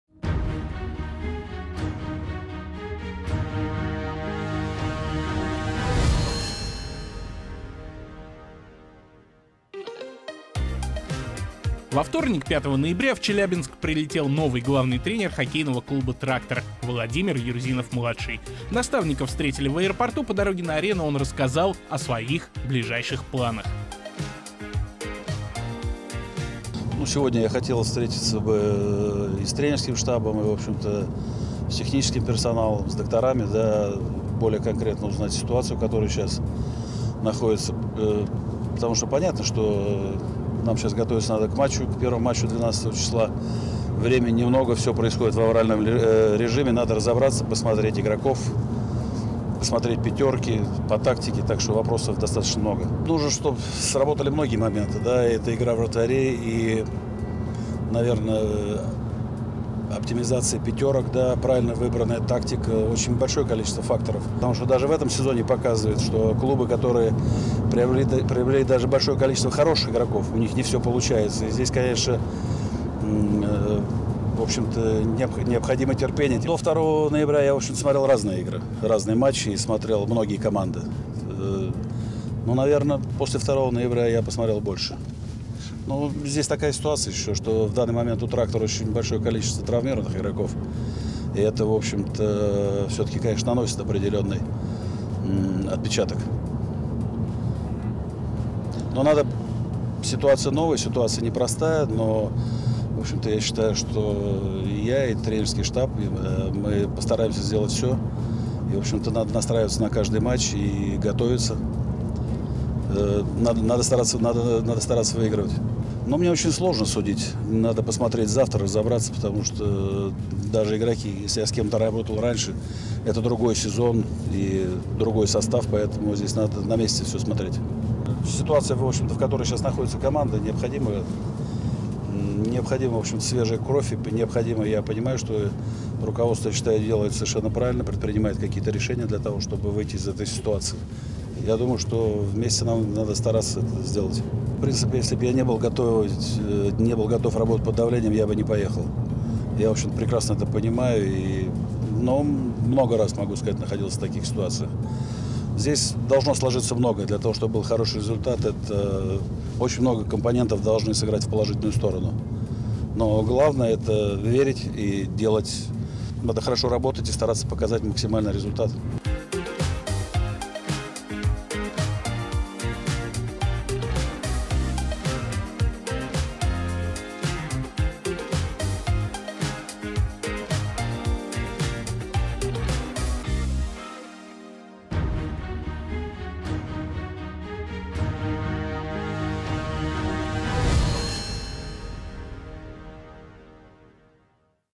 КХЛ событие – Приезд Владимира Юрзинова-младшего в Челябинск
Во вторник 5 ноября в Челябинск прилетел новый главный тренер ХК "Трактор" Владимир Юрзинов-младший. Наставника встретили в аэропорту, и по дороге на арену он рассказал о своих ближайших планах...